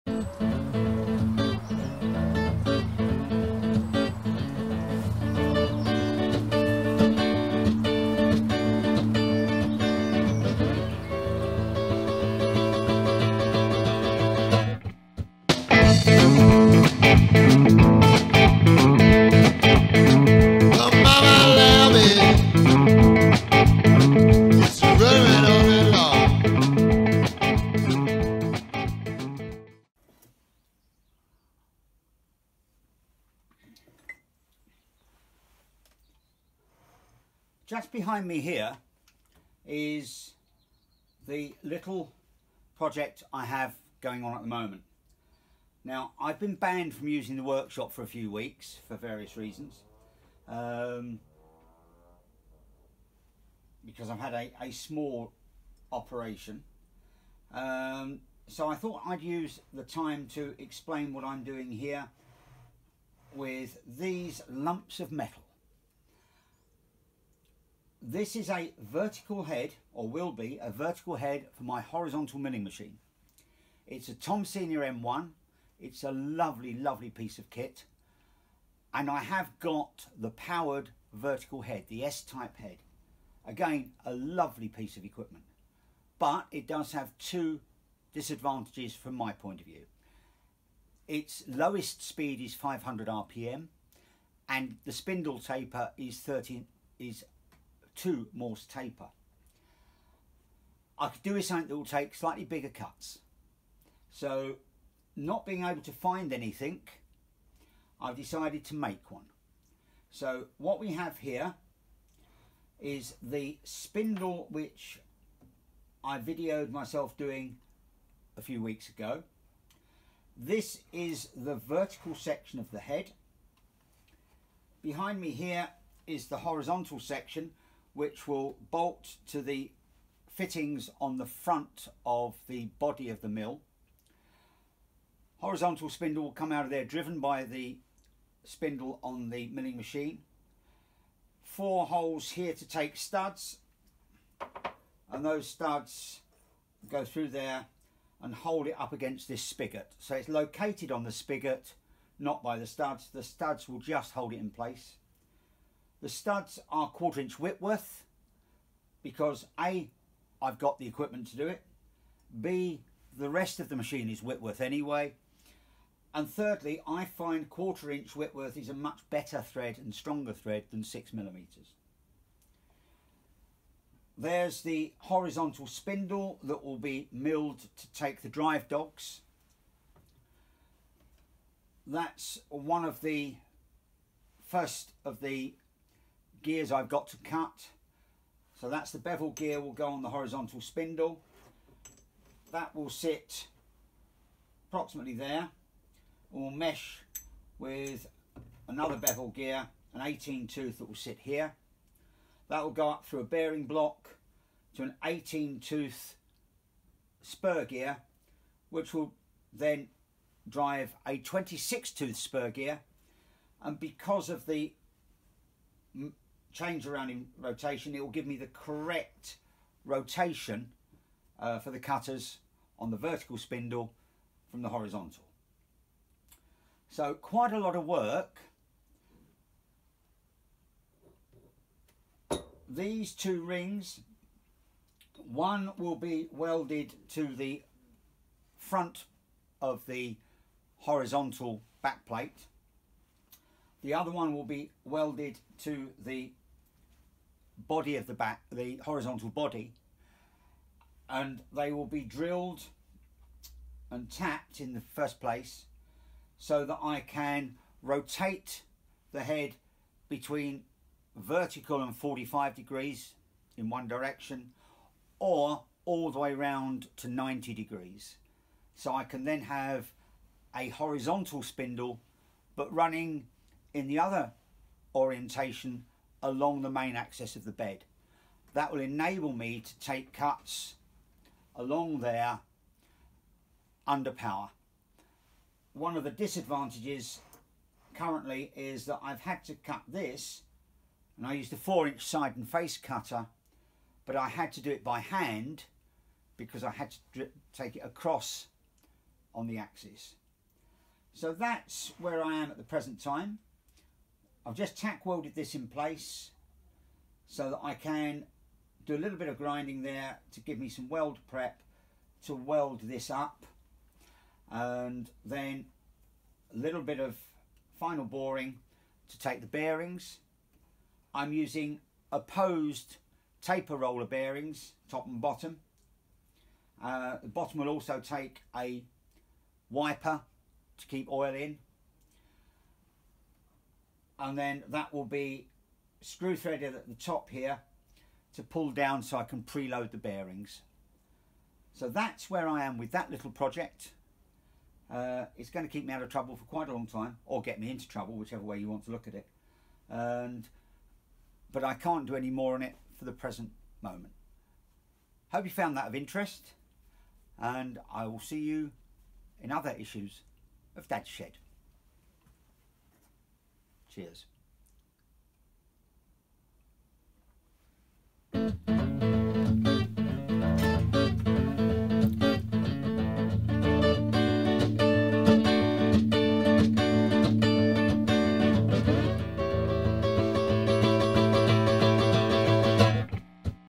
Dads Shed 5 Vertical Milling head project - a short overview!
During an enforced lay off from doing any work, I've put together a short video explaining what I'm doing making a vertical milling head for my Tom Senior Mill that's powered by the Horizontal spindle